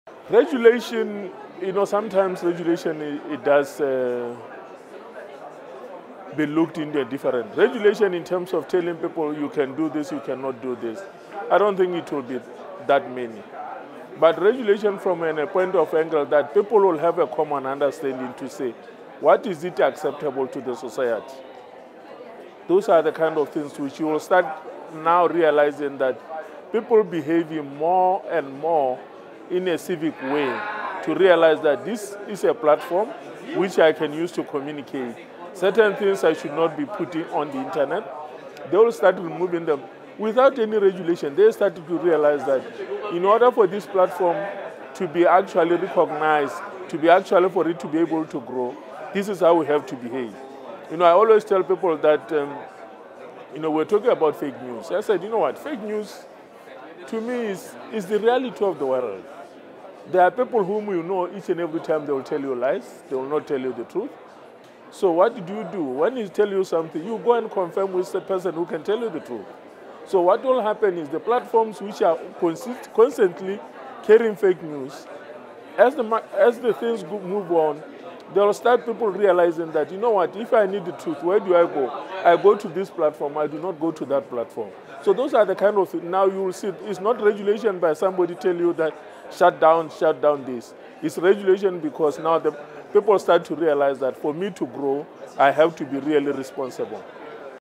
Global IGF 2017: Tshoganetso Kepaletswe on Internet Regulation
Tshoganetso Kepaletswe, deputy-chief executive for Bocra, shares their answer to the question, “Will the Internet become much more regulated in the next 10 years?” Recorded at the Global Internet Governance Forum 2017 in Geneva, Switzerland, by the Imagining the Internet Center, an initiative of Elon University, North Carolina, USA.